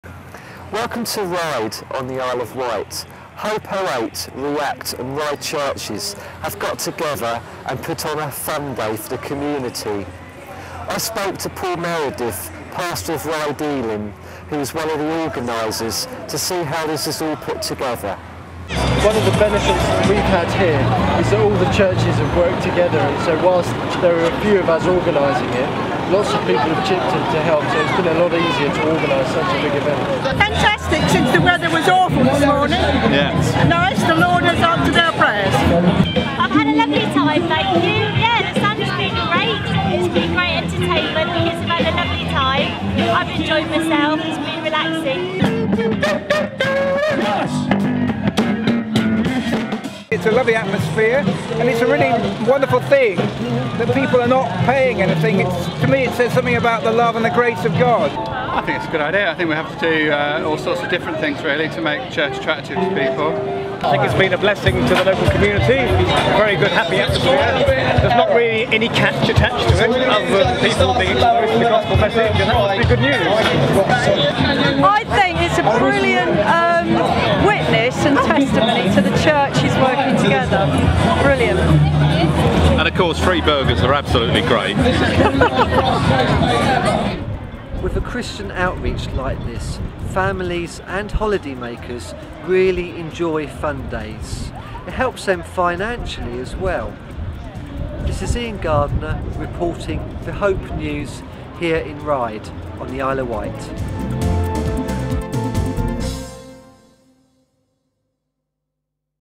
Ryde Funday 08
Do you enjoy funfilled family days? Can't seem to find the pennies?  This year's funday in Ryde, on the wee Isle of Wight, offered something entirely different. Something for everyone. So go on, join the fun and enjoy the ryde!